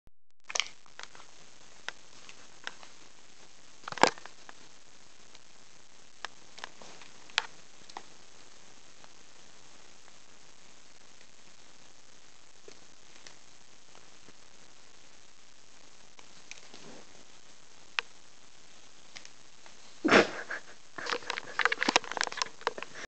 Si gioca con il Tato
Io mentre gioco con il Tato e il telecomando...a volte vorrei riposare ma c'è sempre qualcuno che vuole giocare con me e allora ogni tanto ci metto anche le unghiette in queste giochi hehehe